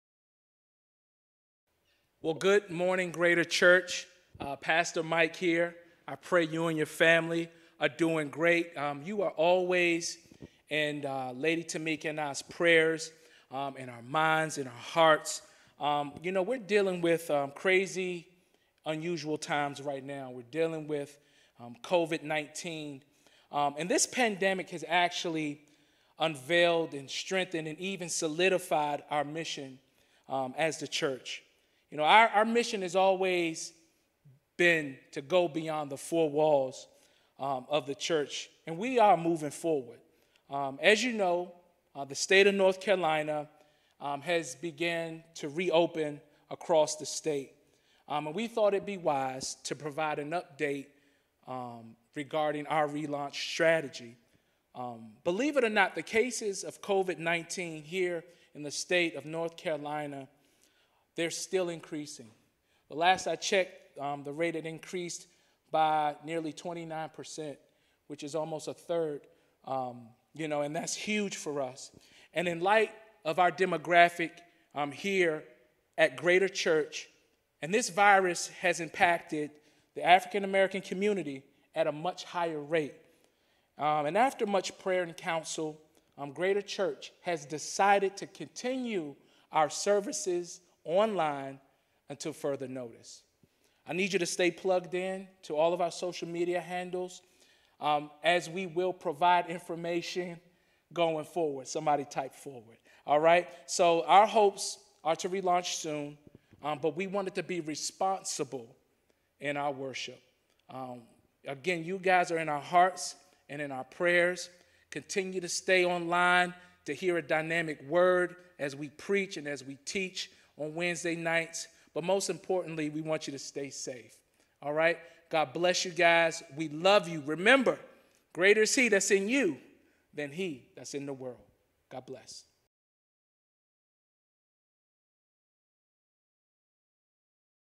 State of the Church - Pastor Mike Roach
A special note from Pastor Mike Roach on the State of our Church as we continue to be "Forward Focused."